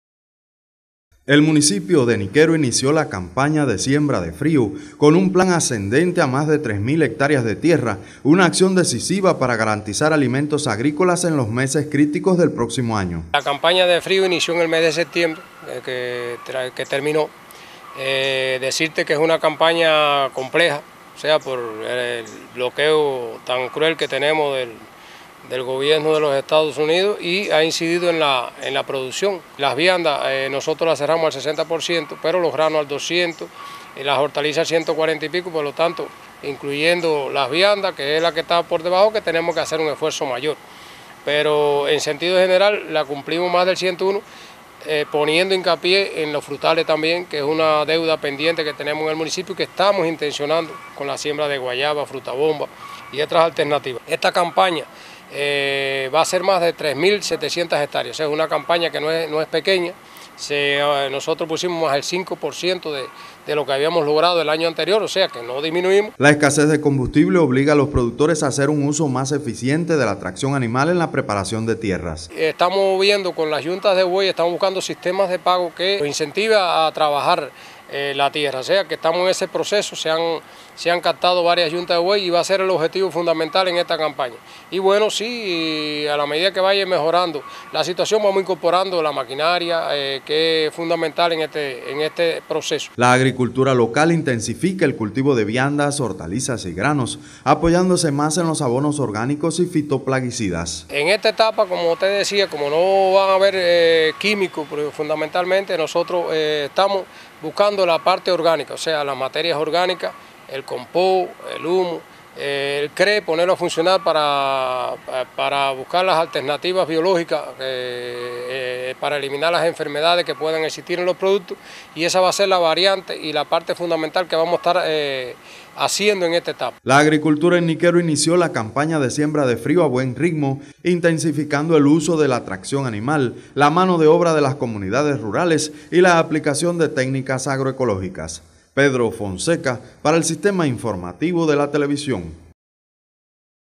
Agricultores niquereños inmersos en campaña de siembra de frio
El sector agrícola en el municipio de Niquero inició la Campaña de Siembra De Frío, intensificando el uso de la tracción animal, la mano de obra de las comunidades y la aplicación de técnicas agroecológicas.